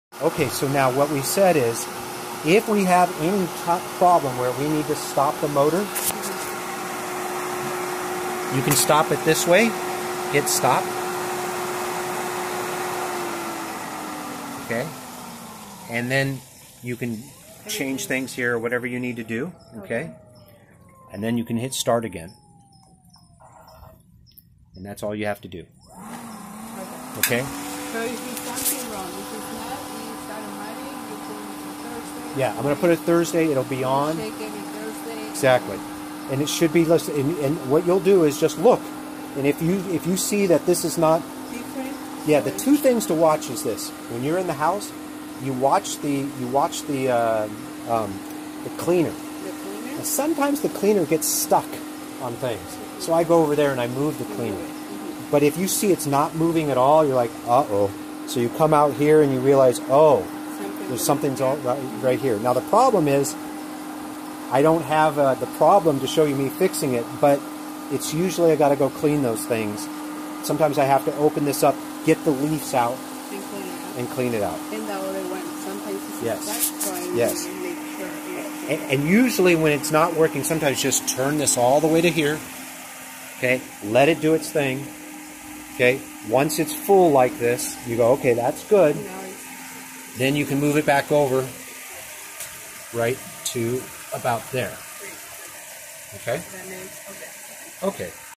Cavendish Pool Maintenance: Starting / Stopping the Pump
How to maintain the Cavendish swimming pool.

To turn on an off the pump manually.